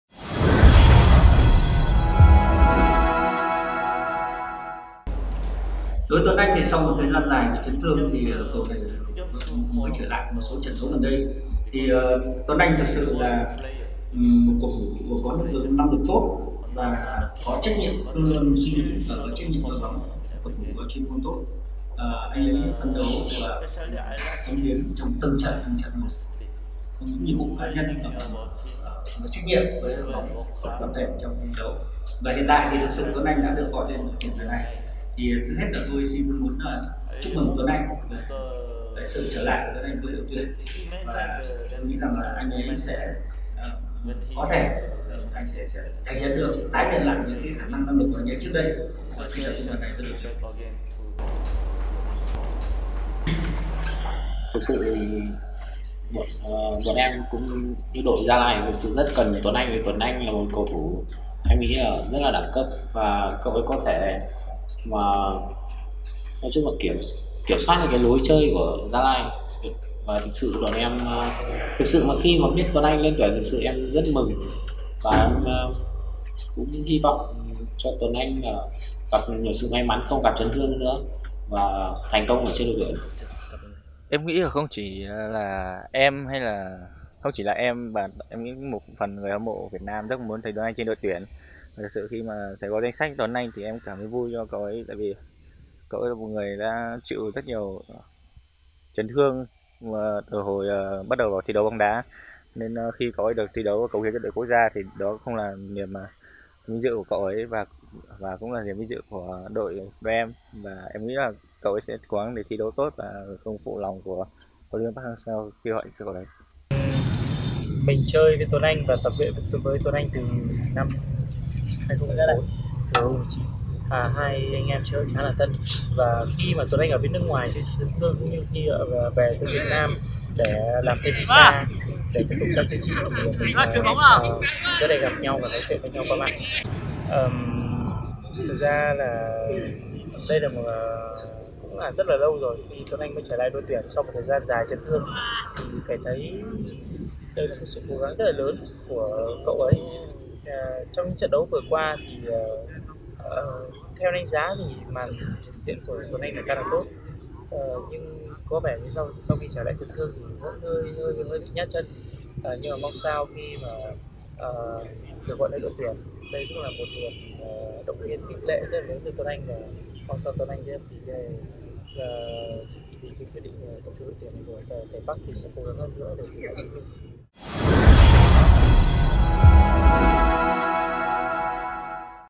HLV Lee Tea Hoon, Văn Toàn, Đức Huy nói gì khi biết tin Tuấn Anh trở lại ĐTQG | HAGL Media
HAGL Media | HLV Lee Tea Hoon, Văn Toàn, Đức Huy nói gì khi biết tin Tuấn Anh trở lại ĐTQG